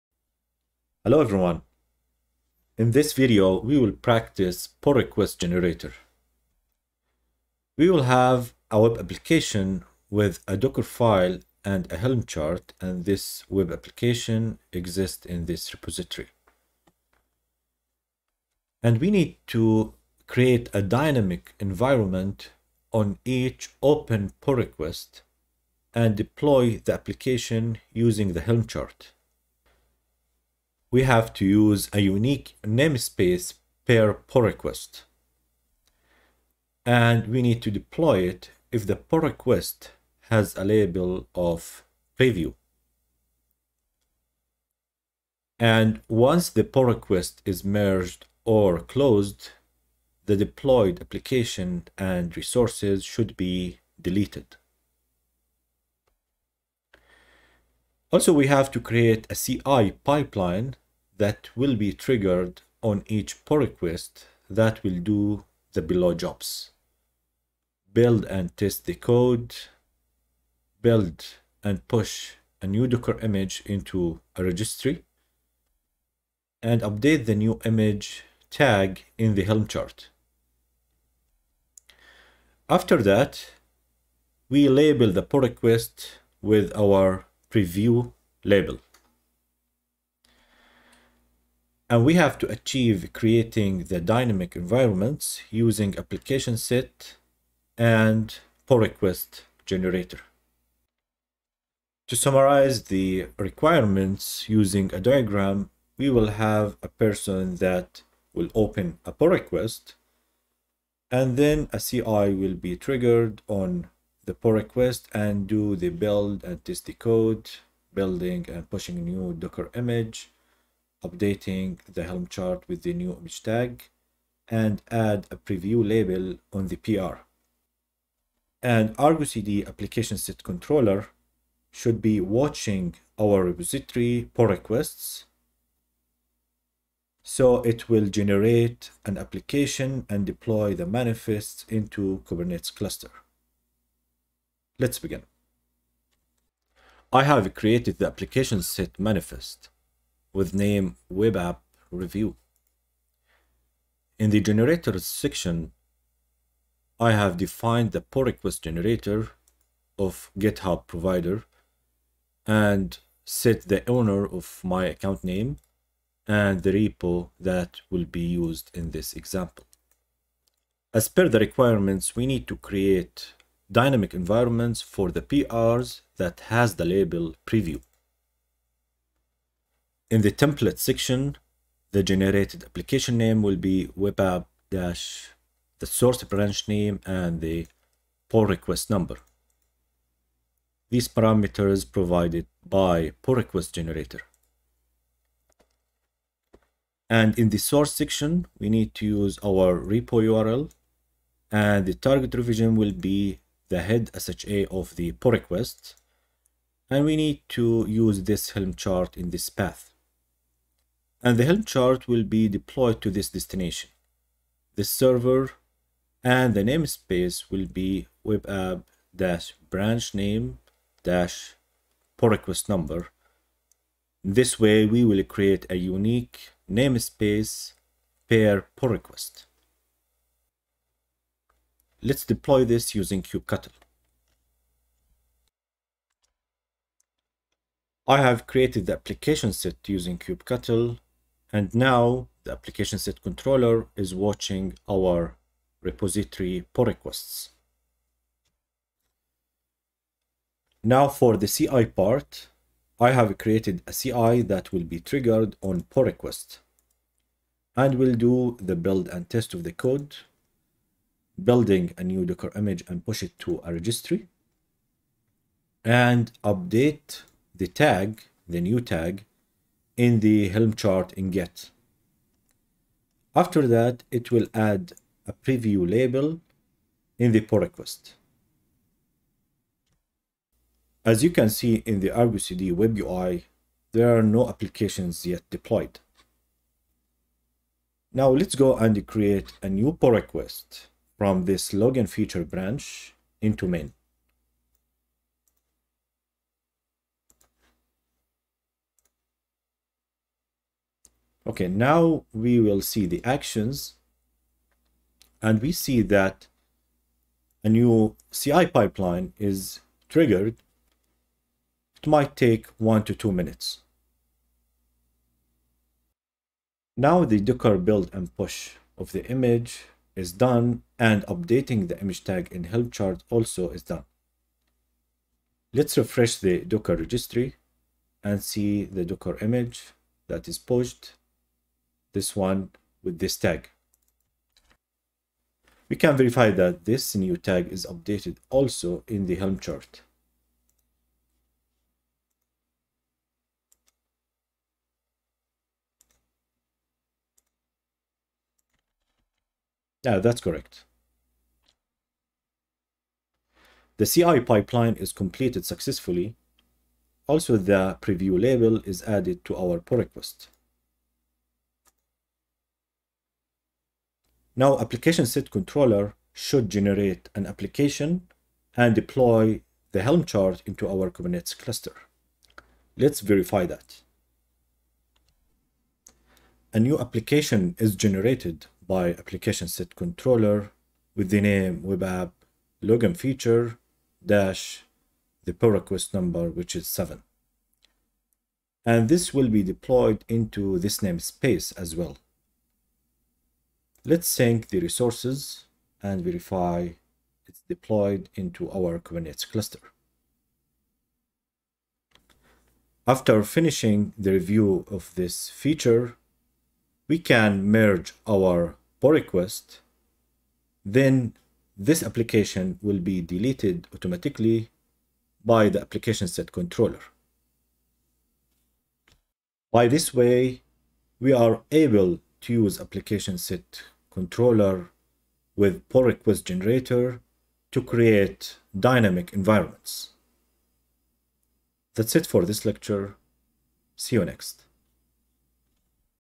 Argo CD | dynamic environments for pull requests using application set
Argo CD application set can create dynamic environments for pull request and once pull request merged or closed, environment will be deleted.